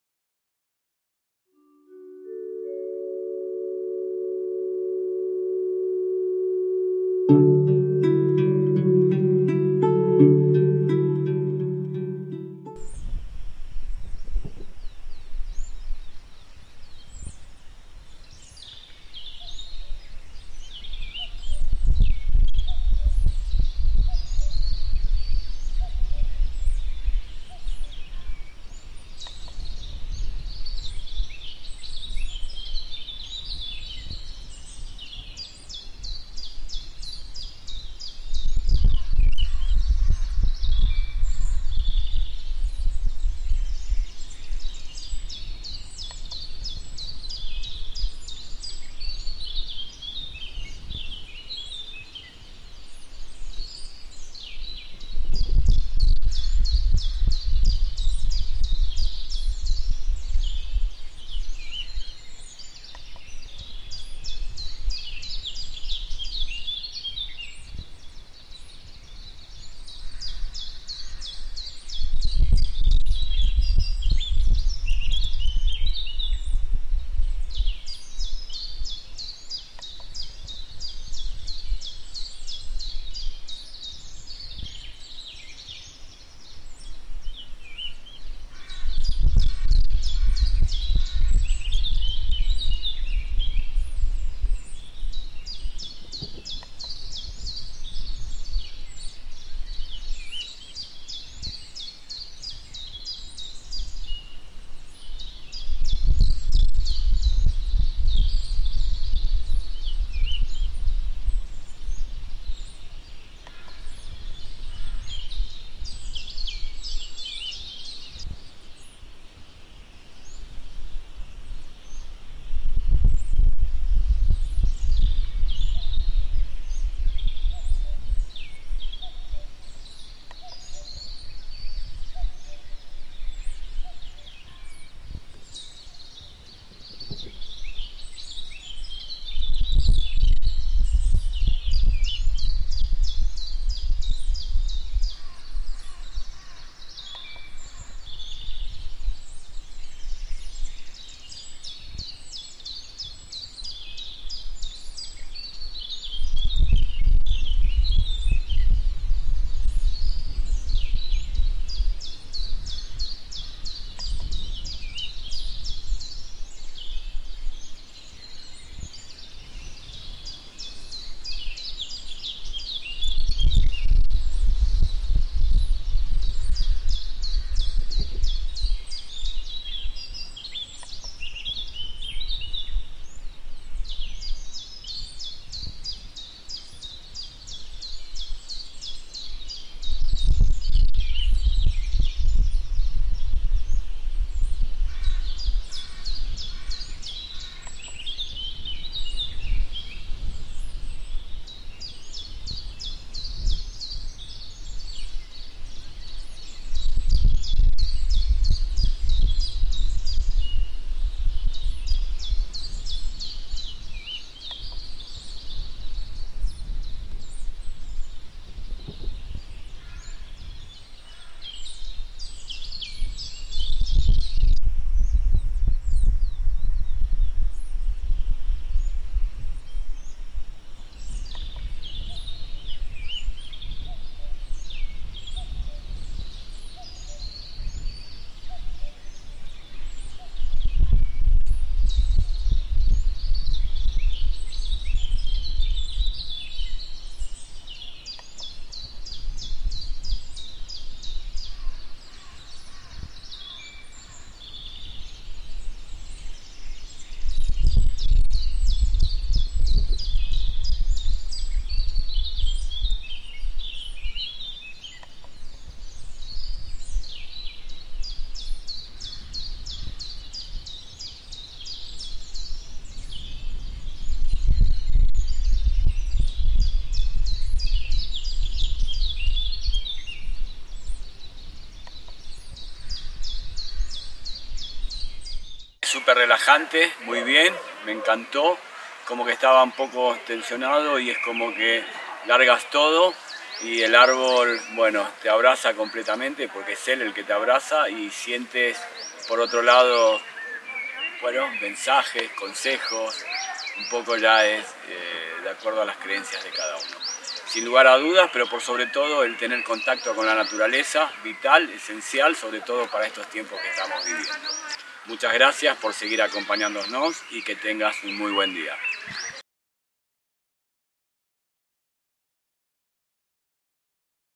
ARBOTERAPIA | CONECTANDO con la ENERGÍA de los ÁRBOLES
En este video te enseñamos nuestra manera de abrazar árboles esperamos que encuentres la tuya.